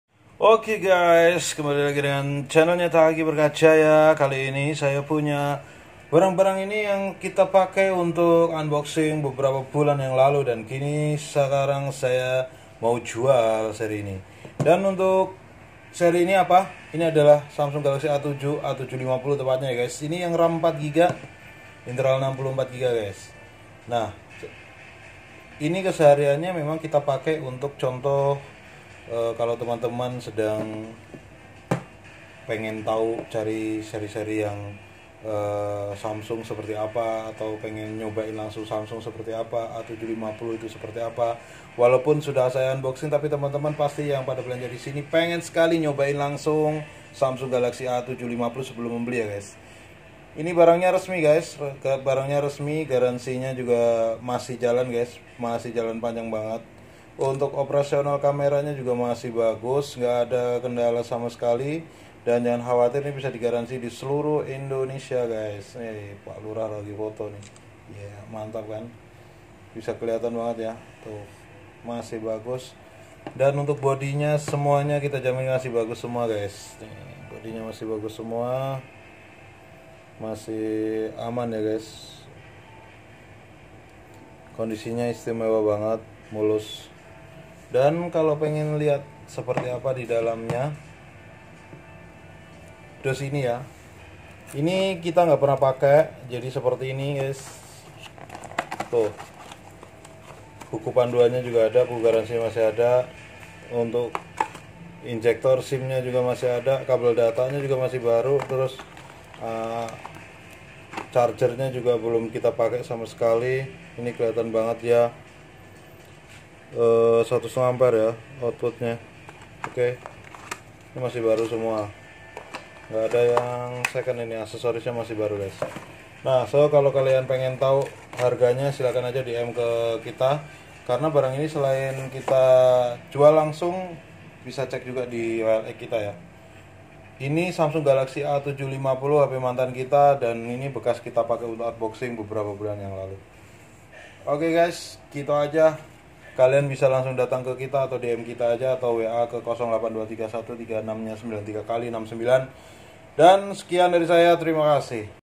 Jual murah GALAXY A750 BEKAS unboxing
Ini saya jual murah aja 2,9jt bisa pake Bukalapak atau datang ke toko langsung ya gaesss

rekaman menggunakan camera OPPO F11 PRO